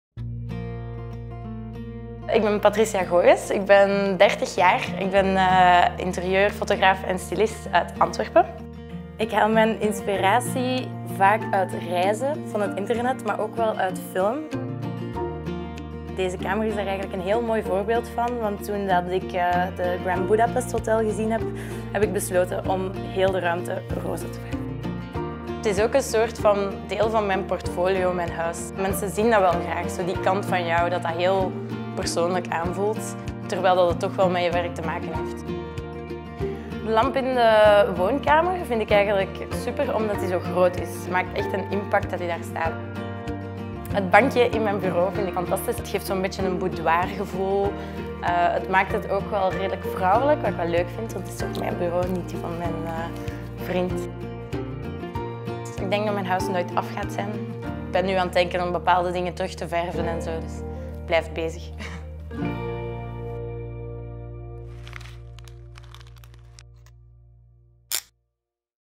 Home Tour: Patricia Goijens - enter the home behind Europe’s most popular bathroom
What do you get if you cross an interior stylist, an architect and The Grand Budapest Hotel? A marble-clad home with a Wes Anderson-inspired colour palette, and a Pinterest-famous bathroom. And that’s no joke. Interior stylist and photographer Patricia Goijens showed us around her 135-metre Antwerp flat which she designed with her architect fiance, Dieter. Take the tour and meet their British Shorthair, George the Furst